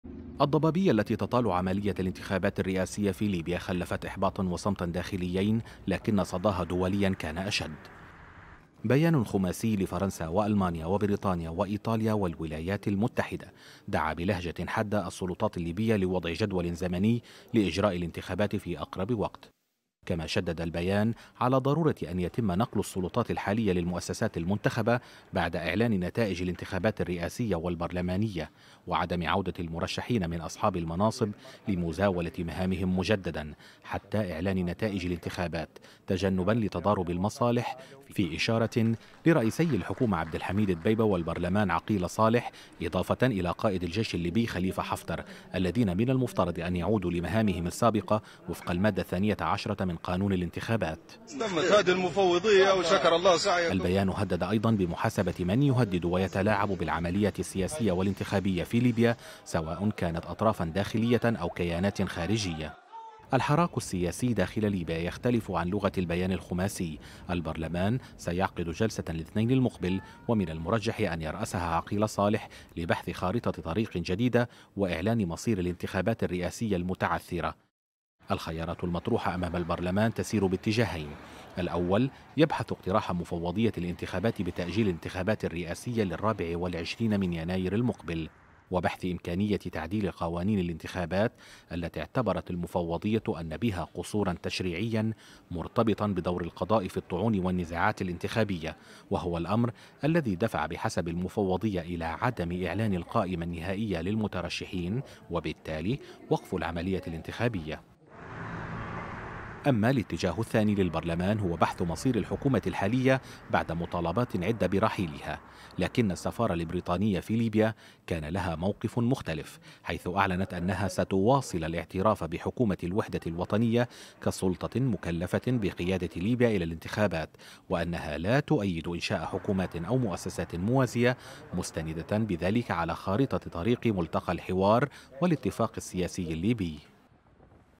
بيان خماسي غربي يهدد معرقلي انتخابات ليبيا
بعد تأجيل الانتخابات..بيان أميركي بريطاني ألماني فرنسي إيطالي يهدد بمعاقبة كل من يعرقل سير الانتخابات في ليبيا